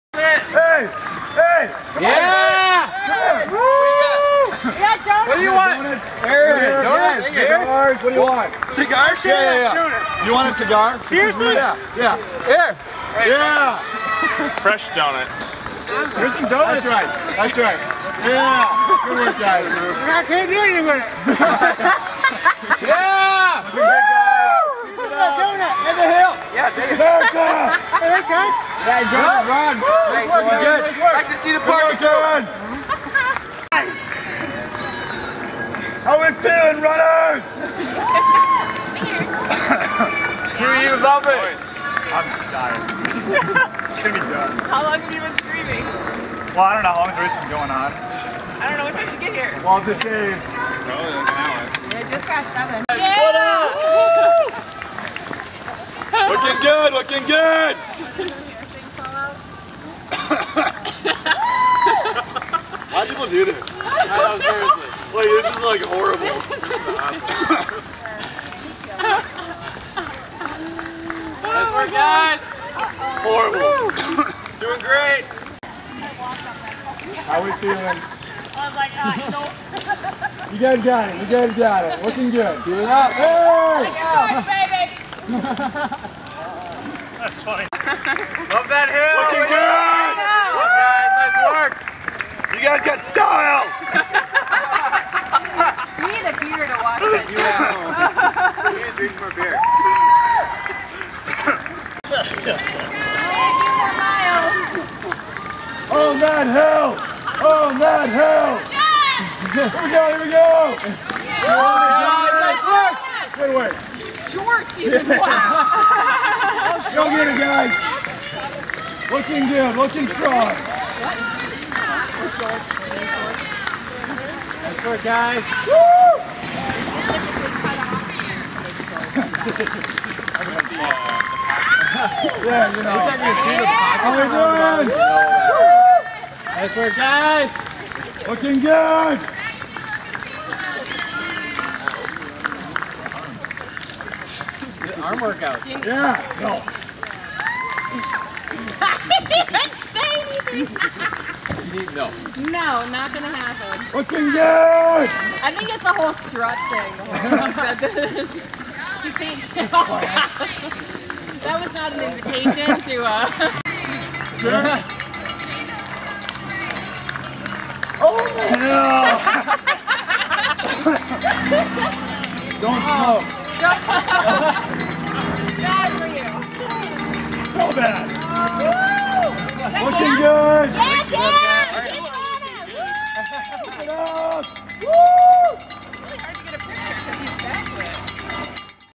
'Murica! Passing out cigars and donuts at the Red, White and Boom half marathon
Cheering the runners for the Red White and Boom July 4 half-marathon while wearing a midriff-showing tank top and cutoff short shorts.